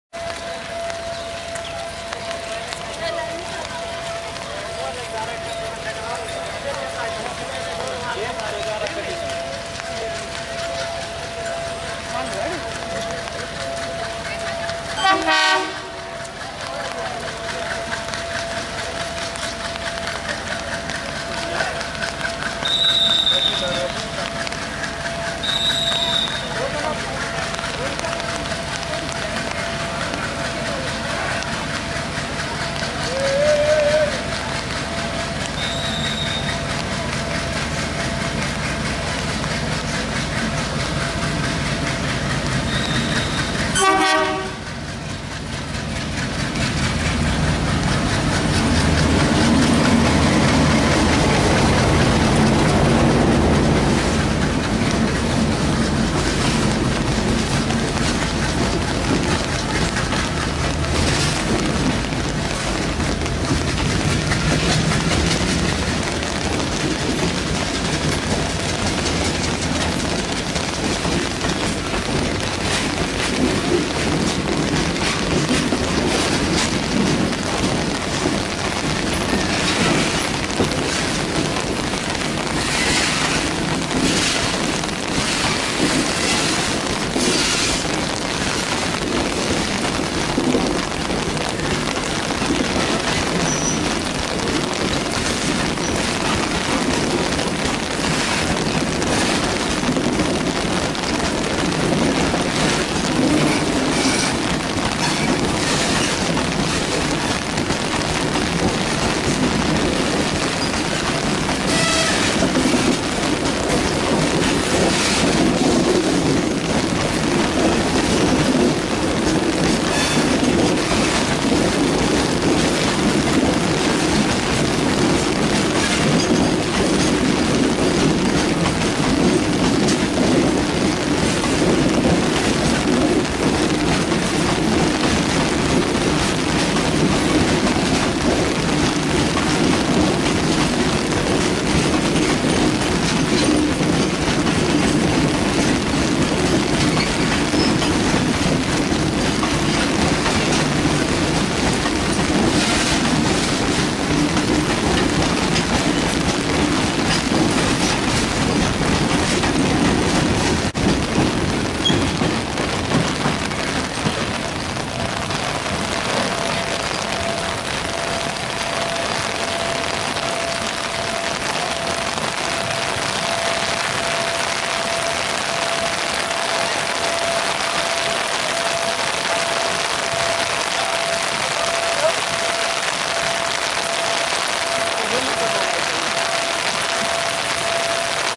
6530 Series Locomotive Donated By The Government Of India Pull Thirty five Wagon In The Rain.
6530 Freight Train Pulling Thirty Five Wagon // This Video Was Captured From Poradaha Junction, Kushtia District, Bangladesh // Broad Gauge Railway.